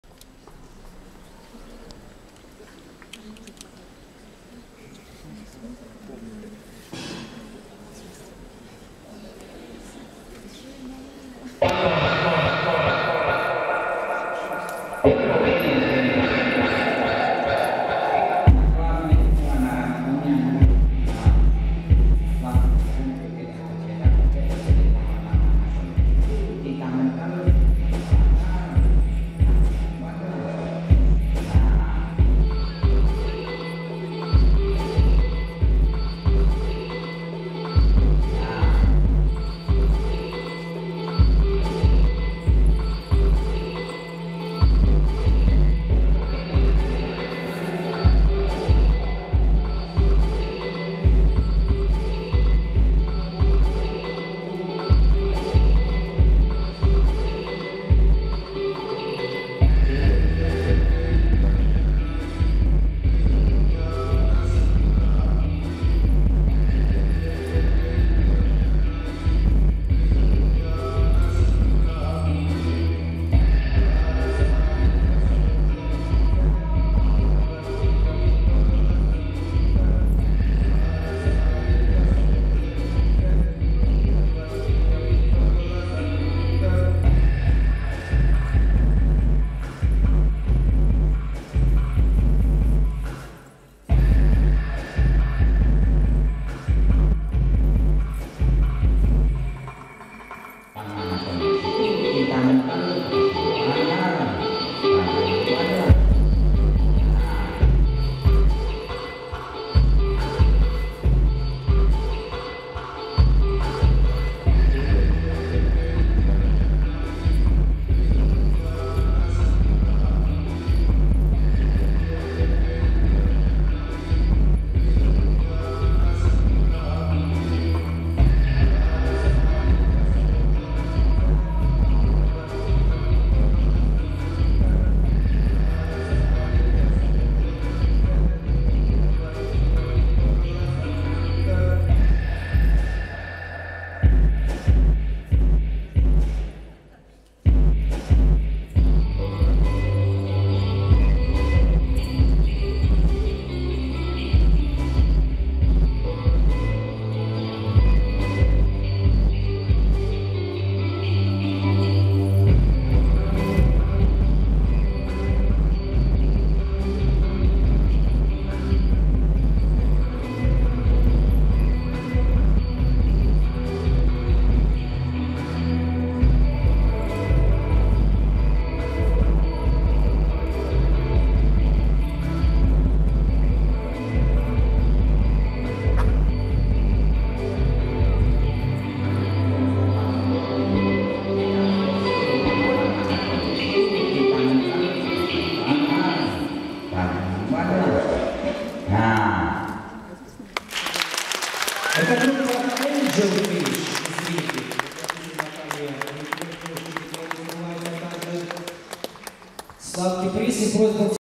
Angelfish studio @ Piramida fest`2012
Choreography: Natalija Krjukova/Angelfish [LV]

Our first time on stage as a studio! The video could have been better, though...